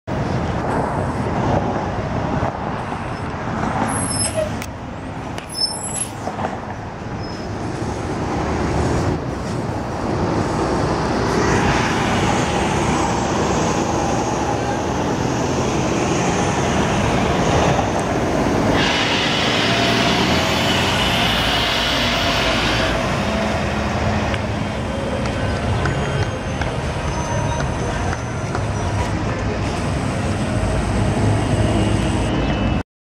2011 Novabus LFSA #5794 / 2013 New Flyer XD60 #4779 on the Bx12 Select Bus Service
Taken 6/12/13 at Sedgwick Avenue And Fordham Road